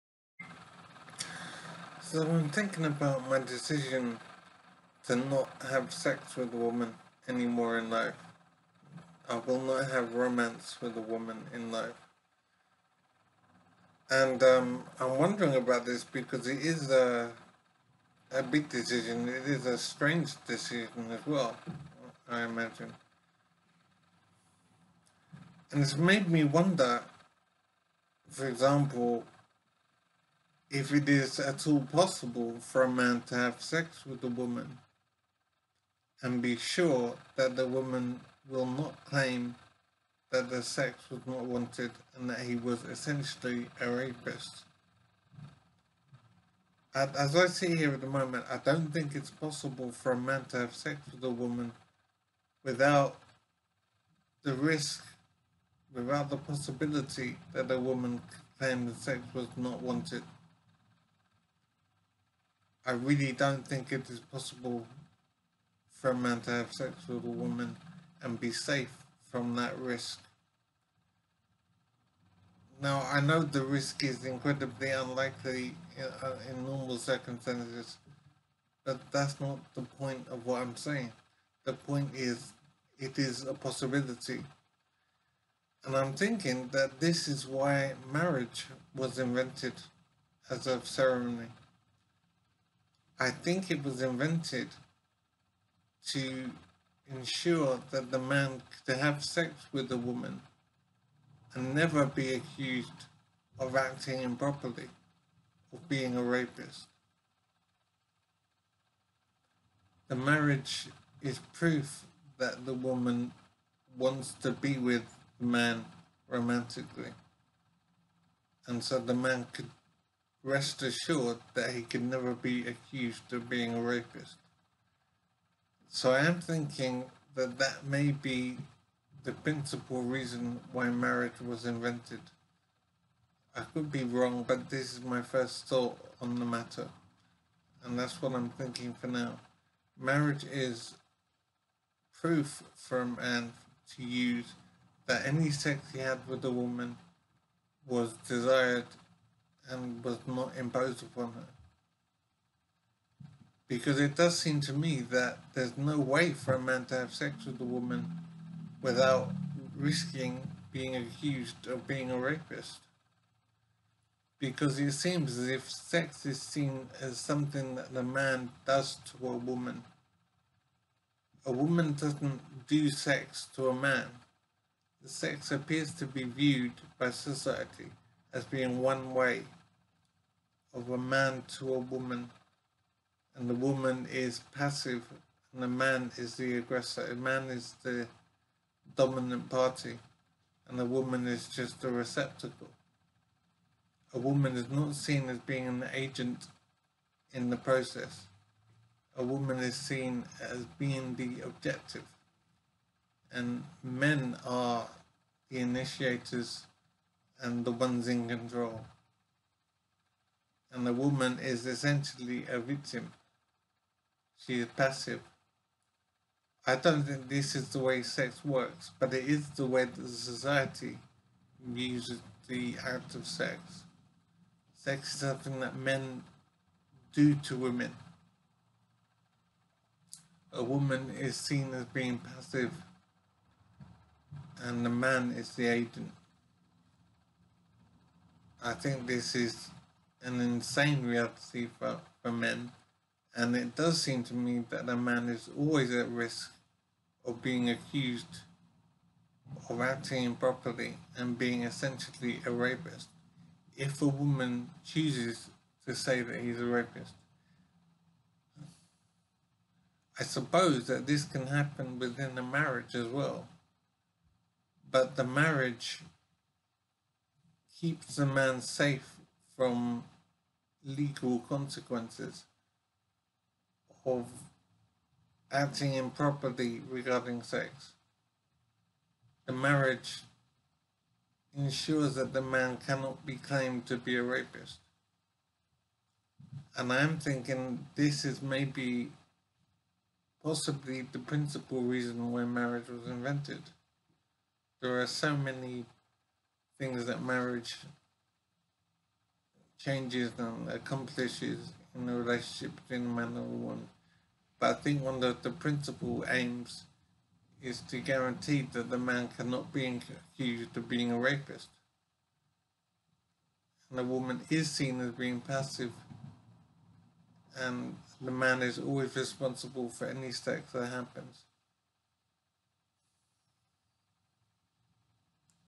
How society views sex and why mariage was invented!
"Sex" is seen in society, as somrthing a man "does" to a woman! The woman is not viewed as being an equal agent in the activity. She is seen as being passive and not having equal control of any sexual activity!
This, inevitably means, that any man having sex with a woman is always at risk of being accused of rape, if she says so! This fact, is possibly why the marriage ceremony was created, in the first place. It is a guarantee of sorts, that any sex that occurs, was wanted by the woman and so protects the man, from any false allegation of rape!